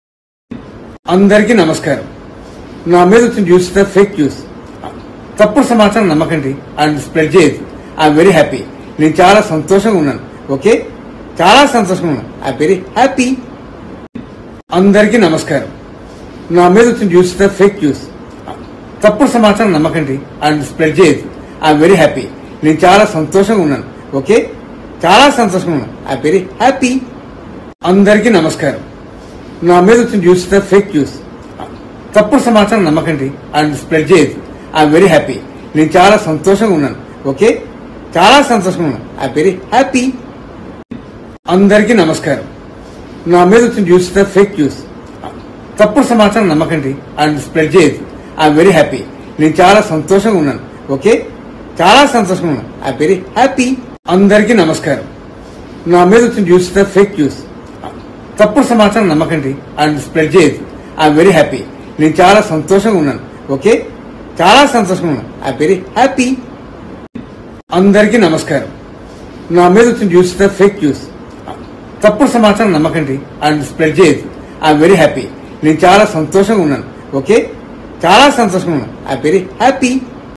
కమీడియన్‌ సుధాకర్‌ ఎమోషనల్ 😥 Comedian Sudhakar Reacts On Fake News About Him
కమీడియన్‌ సుధాకర్‌ ఎమోషనల్ 😥 Comedian Sudhakar Reacts On Fake News About Him  @telugudaily